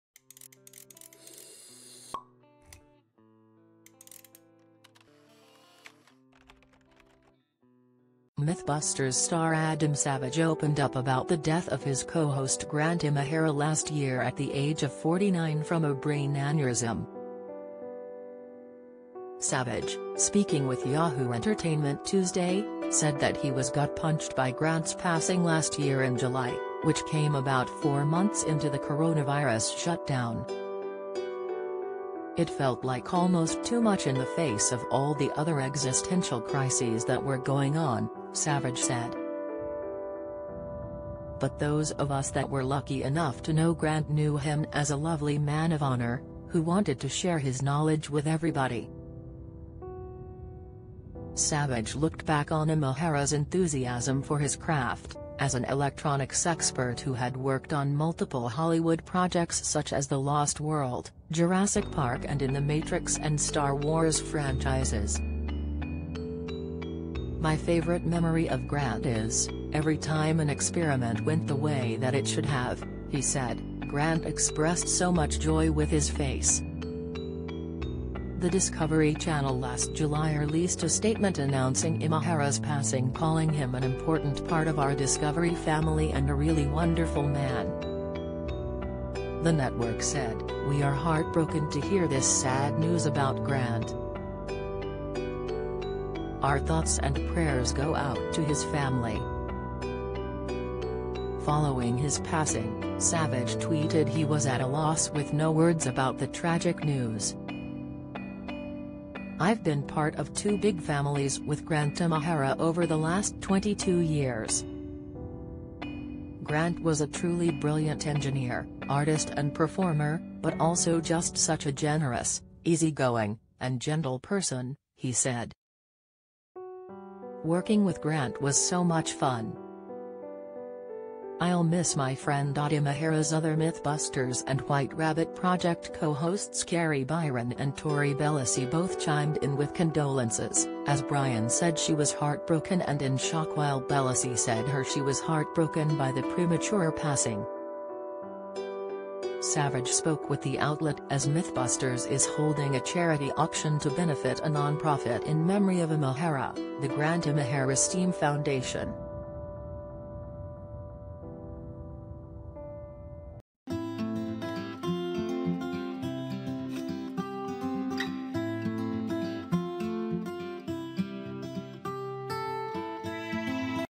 MythBusters star Adam Savage opens up on tragic death of co-host Grant Imahara at 49 last year12 927
MythBusters star Adam Savage opens up on tragic death of co-host Grant Imahara at 49 last year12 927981